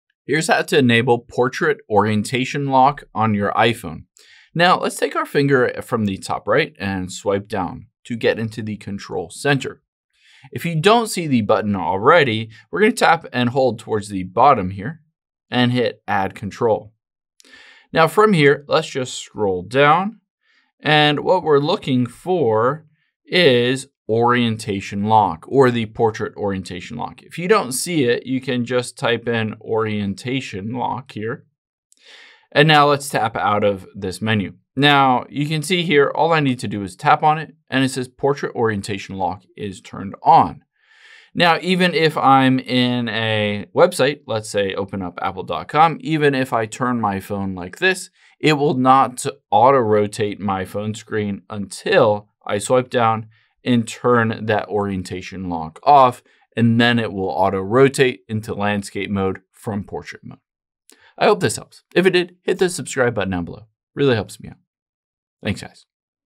How To Enable Portrait Orientation Lock On iPhone
Here's how to lock your iPhone screen to portrait orientation even when turning it to landscape mode.